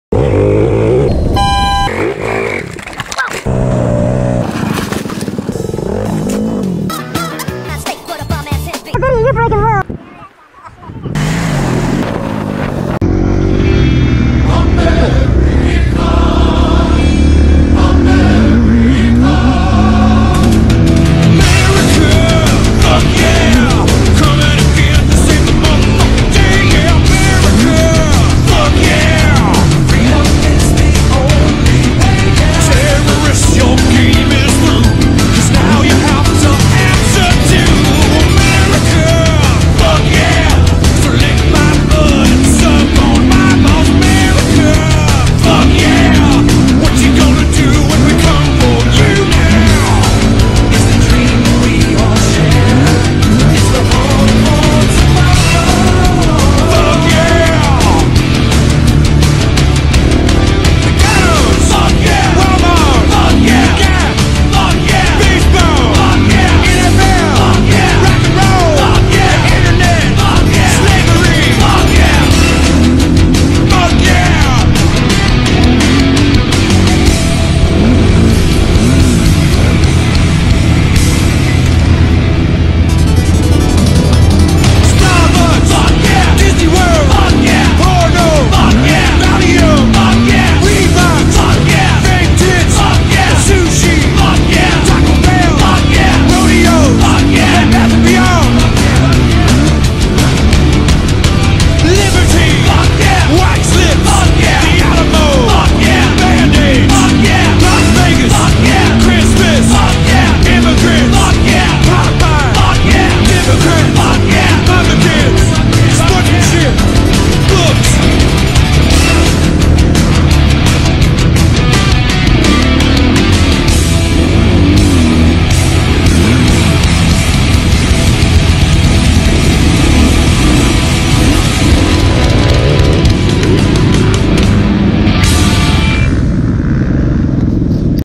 Pala Vet Track after the National
A few changes to the Vet track, and the dirt was really good since it finally cooled off a little bit and overcast enough to stay moist.